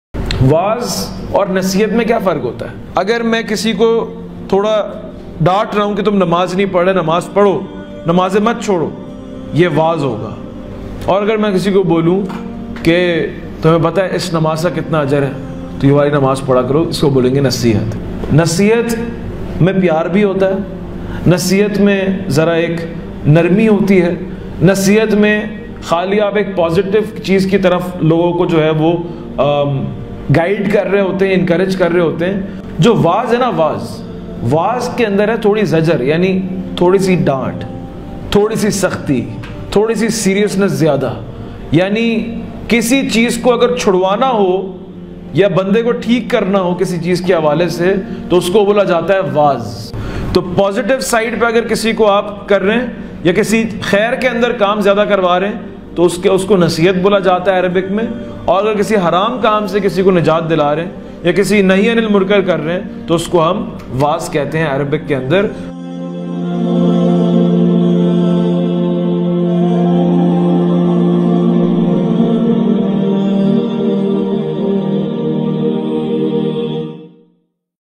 وعظ و نصيحت میں فرق | Motivational Reminder By Ustaaz Adeel Arfeen | Al Midrar Institute
وعظ و نصيحت میں فرق
A Motivational Reminder by Ustaaz Adeel Arfeen

Almidrar.com
FB.com/almidrar
Instagram.com/almidrarinst
Twitter.com/Al_Midrar
Youtube.com/almidrarinstitute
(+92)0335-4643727